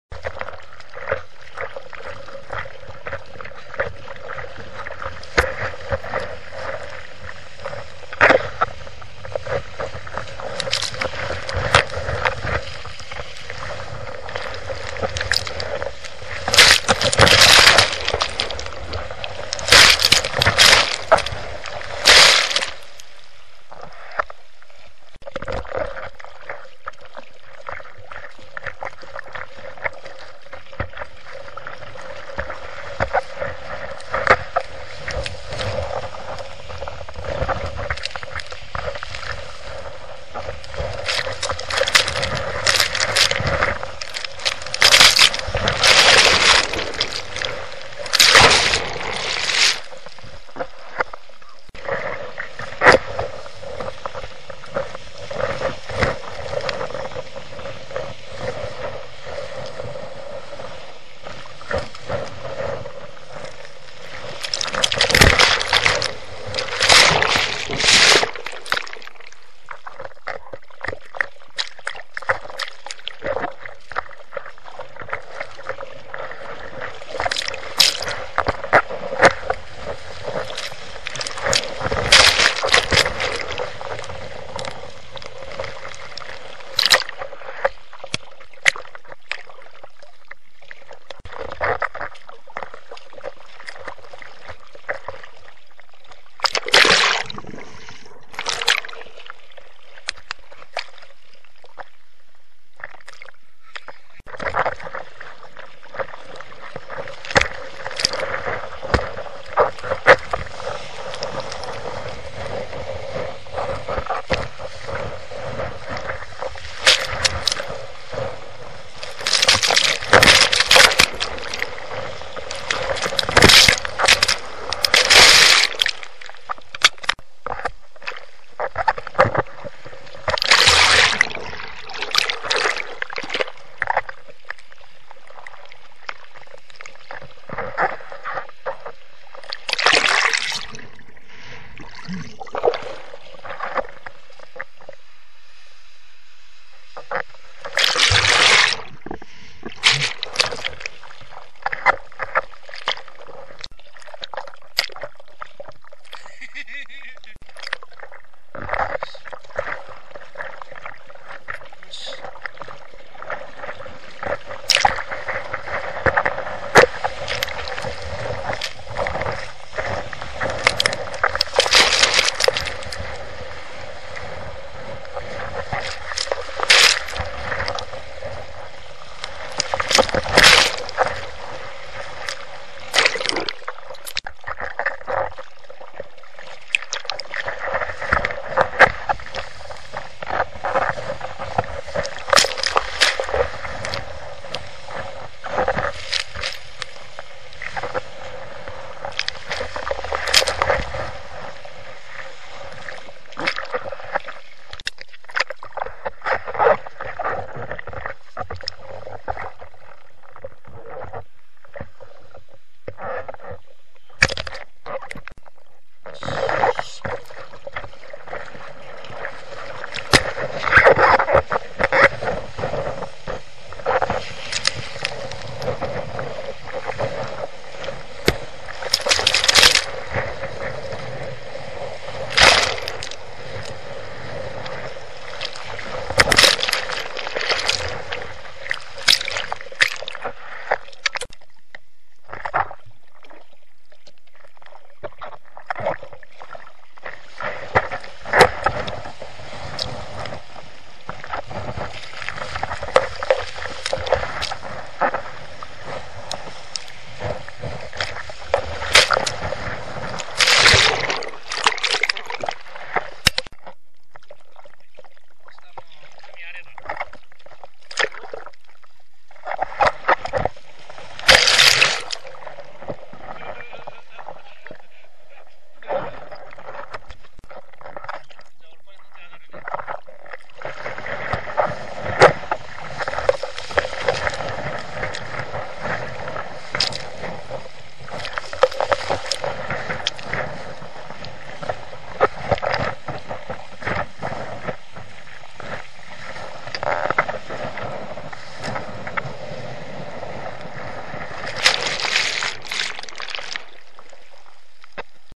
Aee MAGICAM ＳＤ２１ BODY BOARD Ⅱ south ibaraki
AEE magicam SD21 Body board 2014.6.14 south ibarakiⅡ
水陸両用アクションカメラ(ウエアラブルカメラ）AEE magicam SD21でボディボードで水中撮影動画Ⅱ　夕方撮影　スピンやエルロロなどの技もやってみました。べースマウンターを平面マウンターと組み合わせてボディボードに固定して使用。